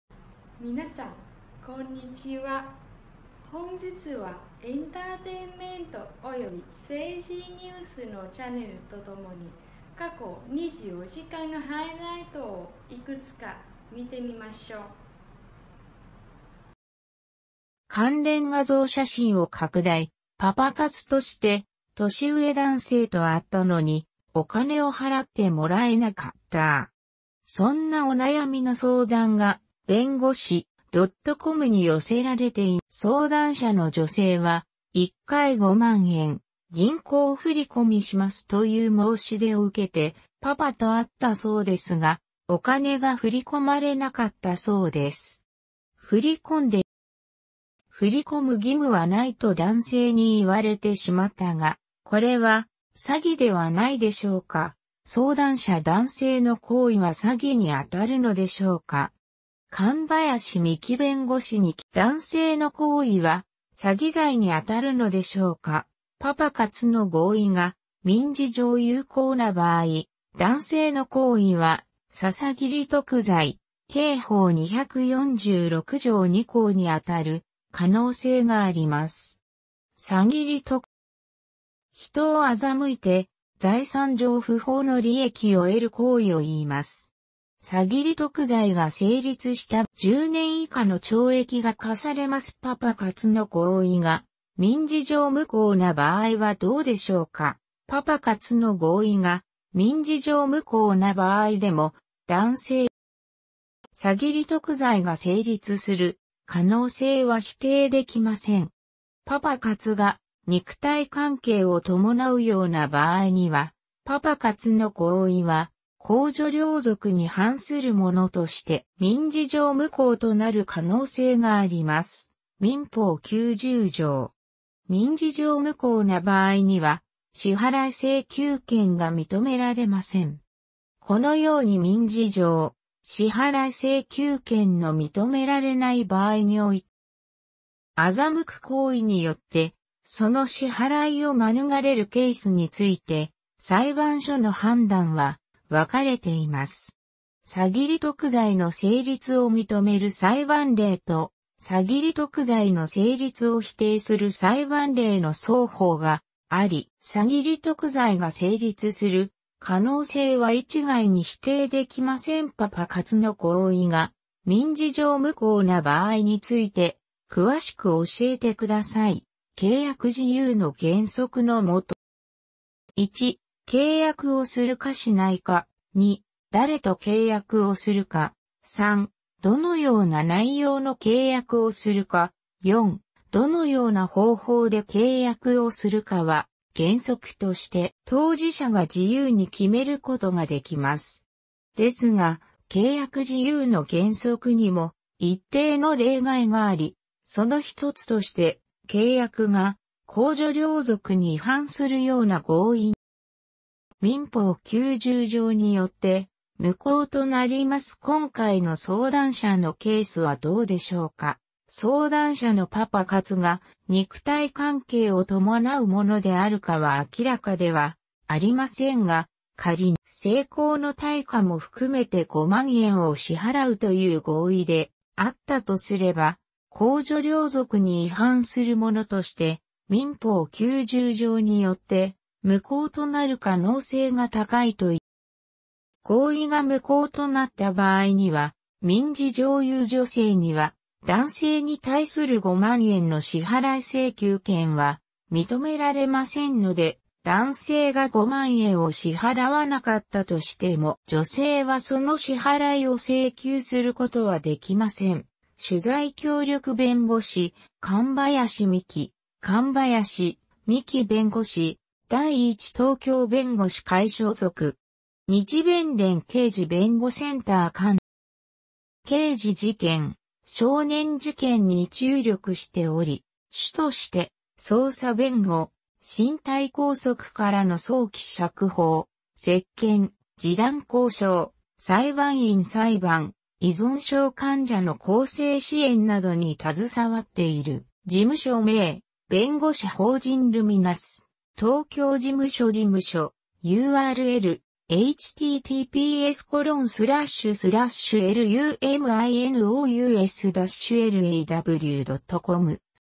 ニュース - 2019年12月02日 パパ活したのに「義務なし」と支払い拒否された「これは詐欺ではないでしょうか？」（2019年12月1日）
関連画像写真を拡大「パパ活」として年上男性と会ったのに、お金を払ってもらえなかったーー。そんなお悩みの相談が弁護士ドットコムに寄せられています。相談者の女性は「1回5万円、銀行振込します」という申し出を受けて、「パパ」と会ったそうですが、お金が振り込まれなかったそうです。「振り込